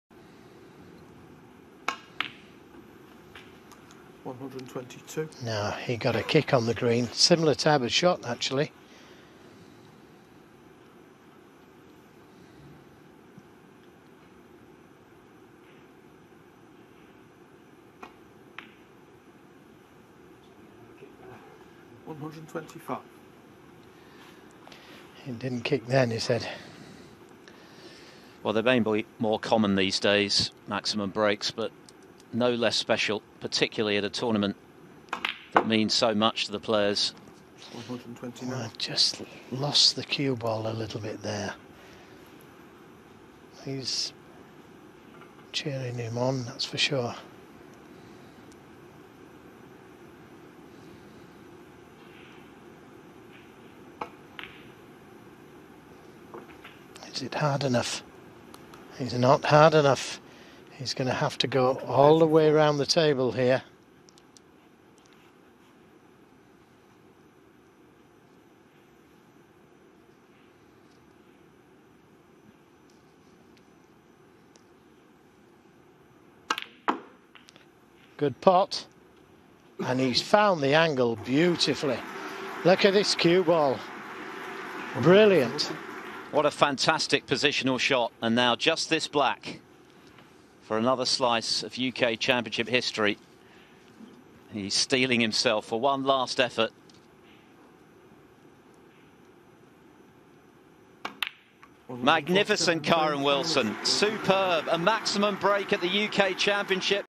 Kyren Wilson makes 147 break in first round of UK Championship
Kyren Wilson fired a maximum 147 break in the sixth frame of his UK Championship first-round match against Ashley Hugill in Milton Keynes.